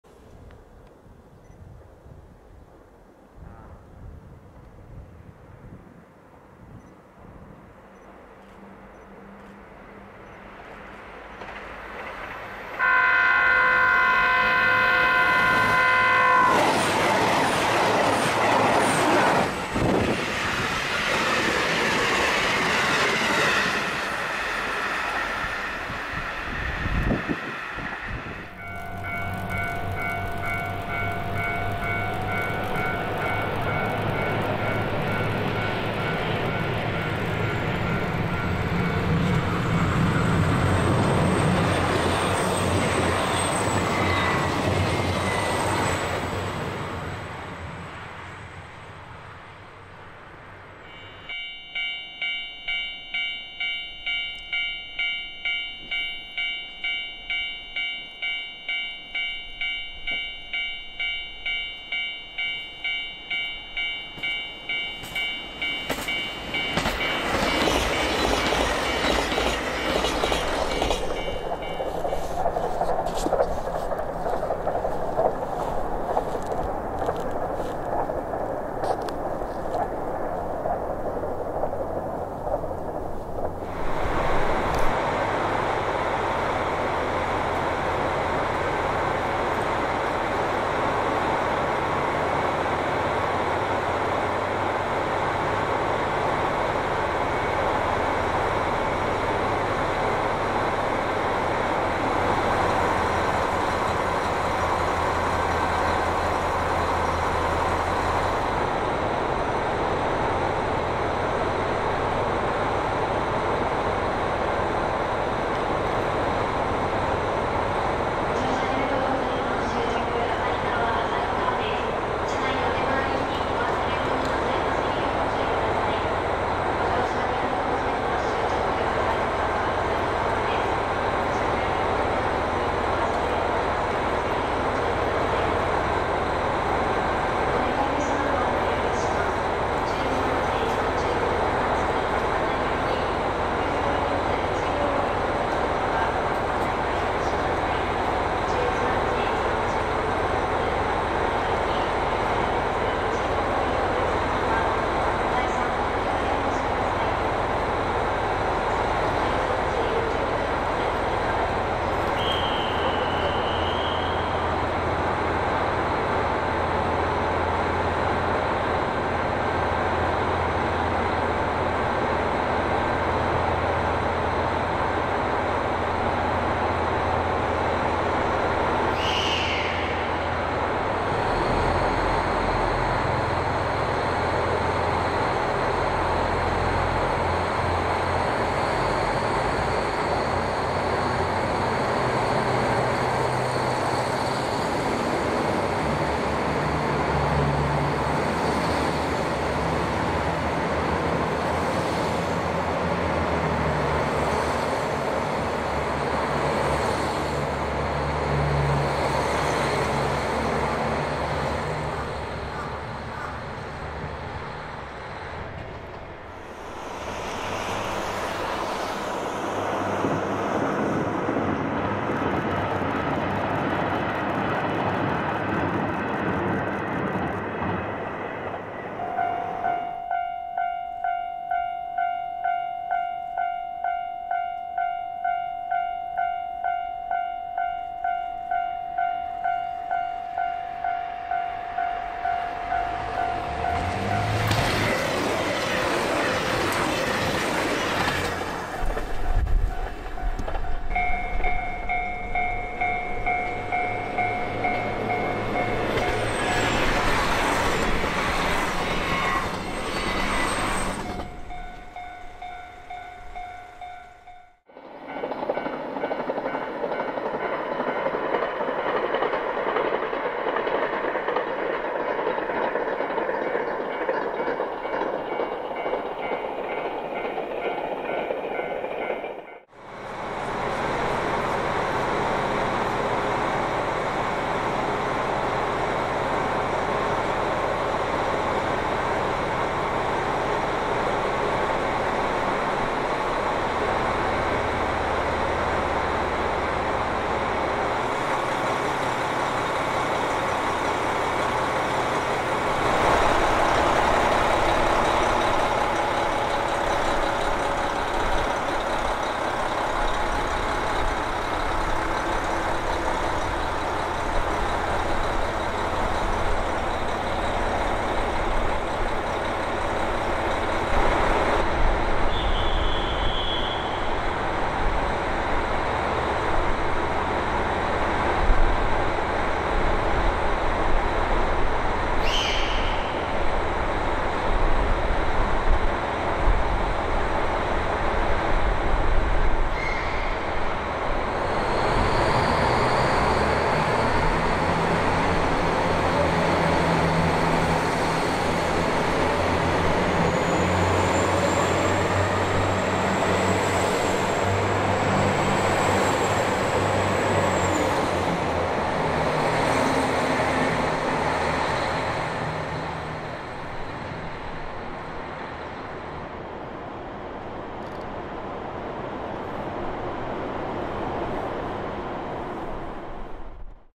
道北のスプリンター･キハ261系0番台（特急宗谷･特急サロベツ）
稚内などの道北方面と 旭川・札幌 を結ぶ キハ261系0番台 を映した短編動画です。
私の住んでいる旭川市近郊の映像が主とした、冬の映像です。
（一部、札幌の”裏菊水カーブ”での 特急すずらん2号 との並走シーンがあります。）

撮影機材：SONY FDR-AX60（ステレオ外部マイク使用）
撮影場所：旭川近郊、一部に札幌市あり
撮影日時：2020年1月